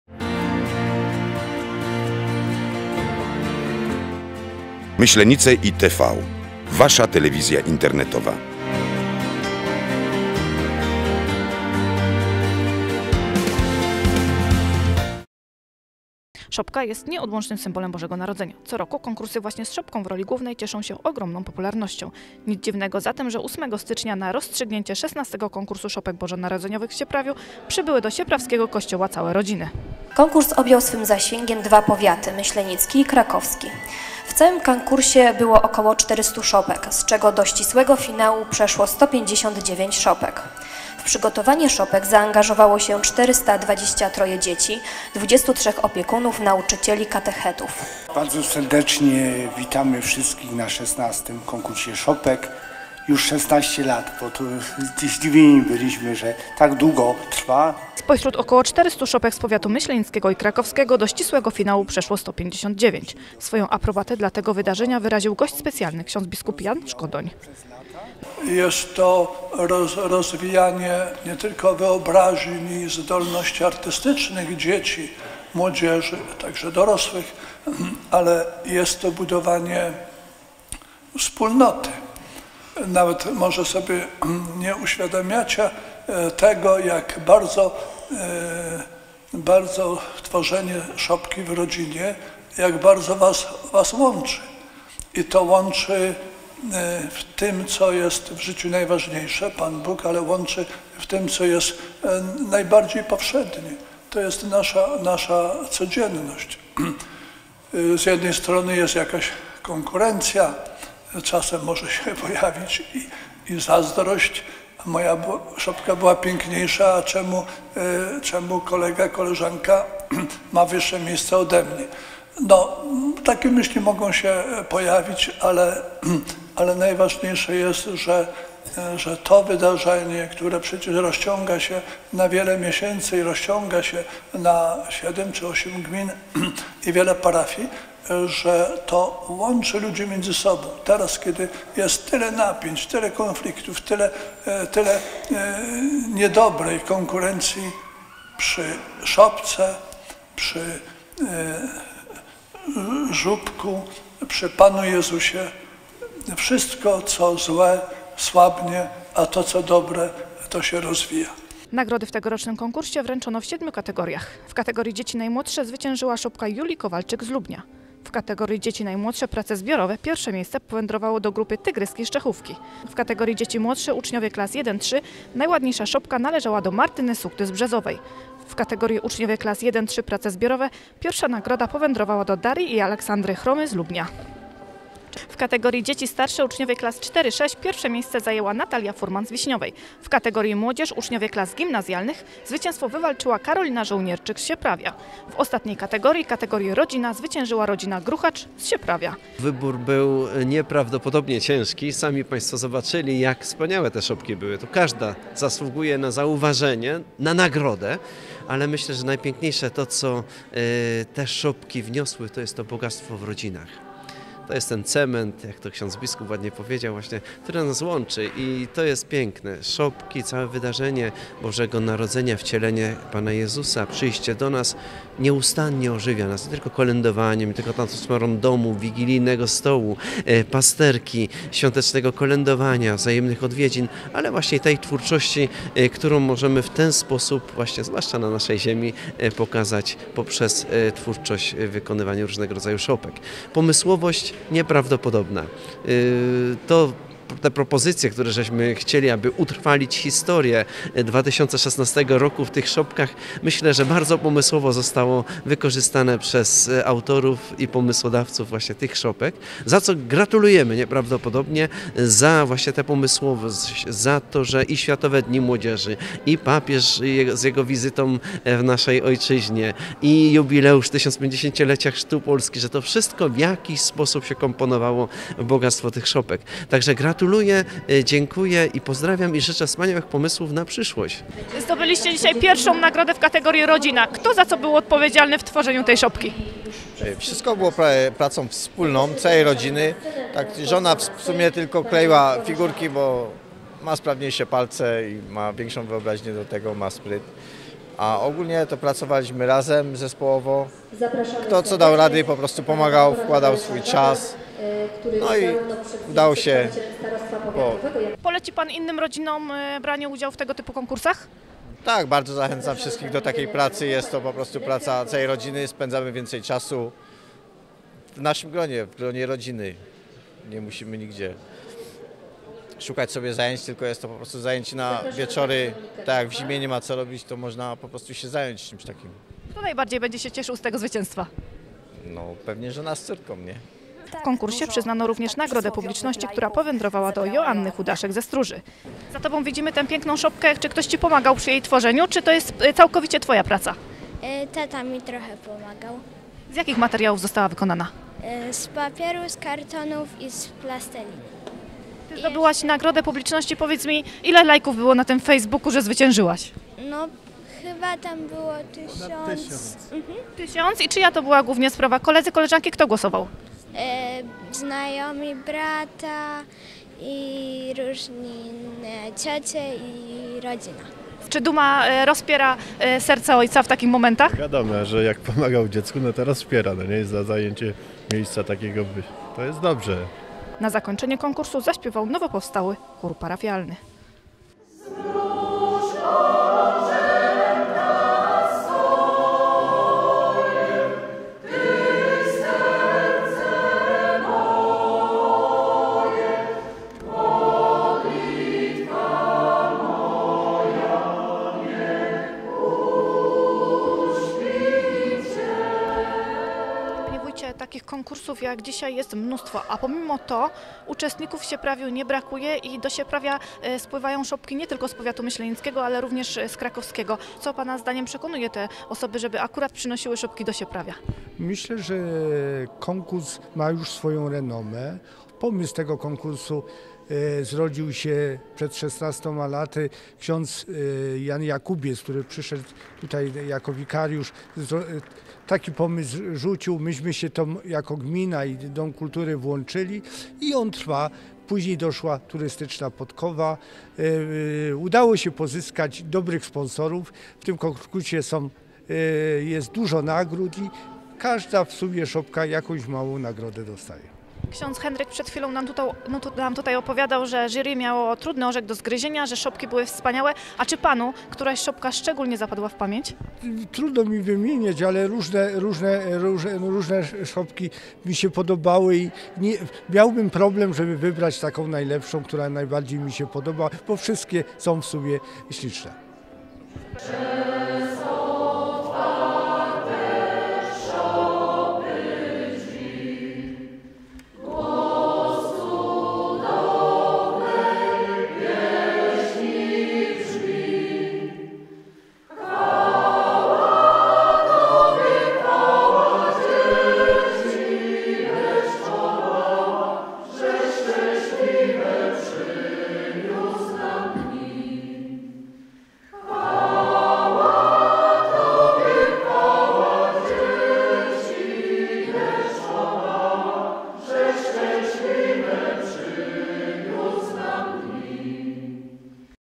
Myślenice iTV: Ponad czterysta zgłoszonych szopek, a wśród nich osiem najpiękniejszych
SIEPRAW: Setki szopek, spośród których trzeba było wybrać kilka najlepszych, taki ciężki orzech do zgryzienia, miało w tym roku jury XVI Konkursu Szopek Bożonarodzeniowych Tradycyjnych i Krakowskich w Sieprawiu. Finał konkursu, ósmego stycznia, zgromadził w sieprawskim kościele twórców i ich rodziny z niecierpliwością czekających na wyniki.